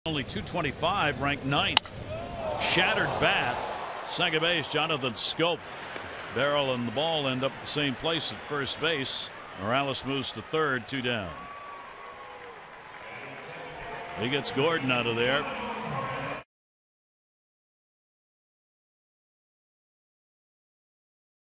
BAL@KC: Gallardo gets Gordon to ground out in 1st
Yovani Gallardo induces Alex Gordon into a soft groundout in the 1st inning